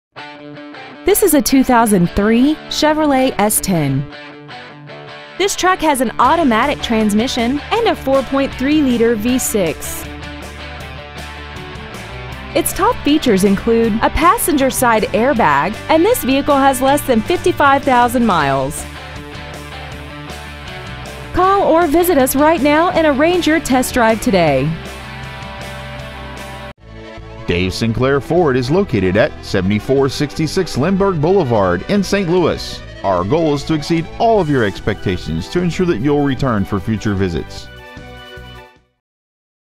2003 Chevrolet S-10 Kirkwood MO
Year : 2003
 Make : Chevrolet
 Model : S-10
 Engine : 4.3L V6
 Trans . : automatic
 Exterior : Light Pewter Metallic - (Gold)
 Miles : 54,395
 Interior : Graphite Cloth
 Stock : 4152701

 7466 Dave Sinclair Memorial Highway

 Pre-Owned 2003 Chevrolet S-10 St. Louis MO